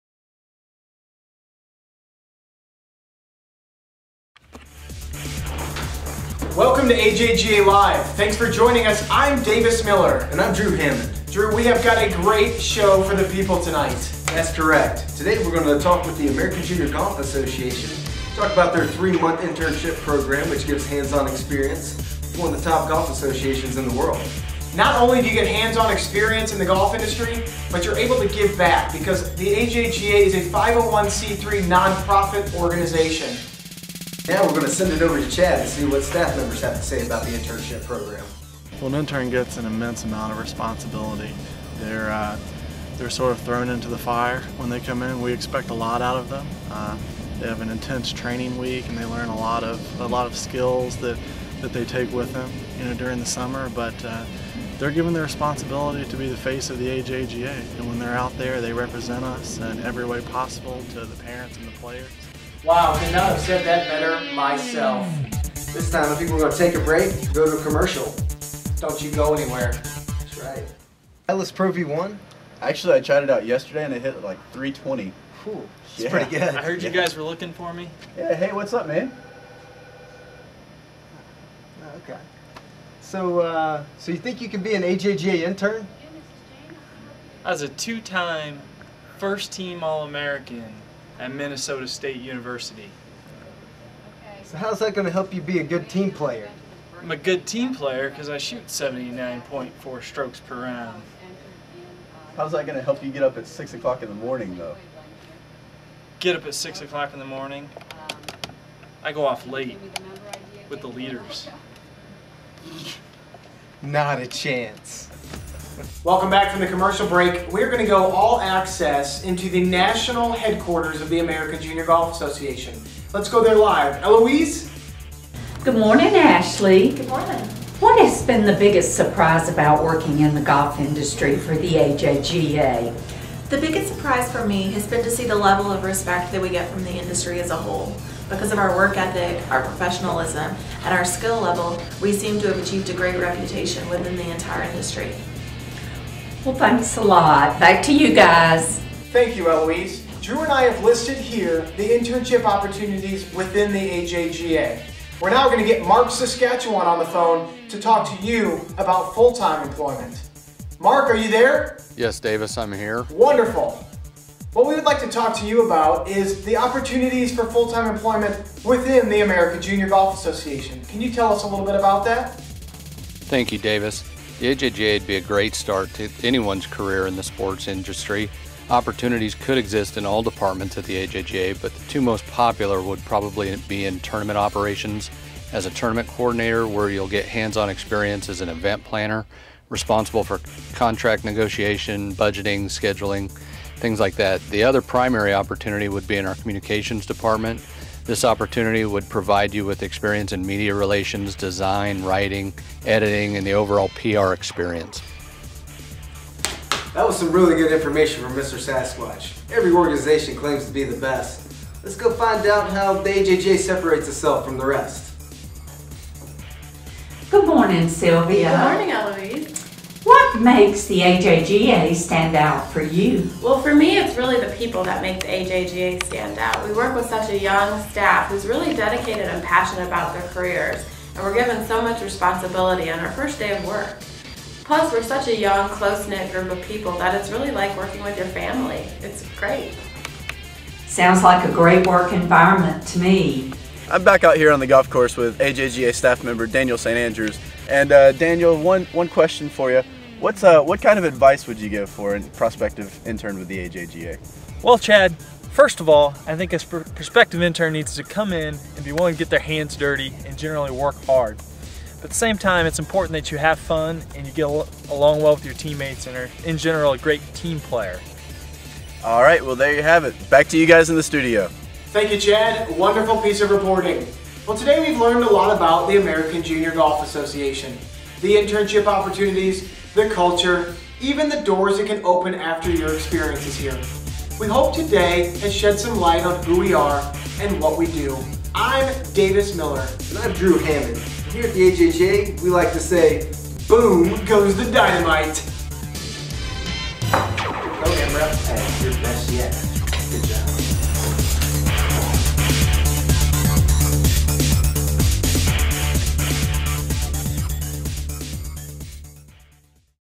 AJGA Film Festival - AJGA Live: Inside the Ropes
2009 AJGA Film Festival.  Storyline: Sportscasters Davis Miller and Drew Hammond take viewers on an all-access look inside what makes the AJGA such a wonderful place to work. Features BEST SUPPORTING ACTRESS Sharon Vinson, and BEST ACTOR NOMINEES Walker Hill and Chandler Sweat. Directed by Chad Smith.

Background: The American Junior Golf Association challenged its staff to create videos explaining why they love working for the AJGA. Divided into 6 teams, the staff created 6 videos on the theme "I am the AJGA." Watch and see why the 56 staff members of the AJGA think it is such a great place to work.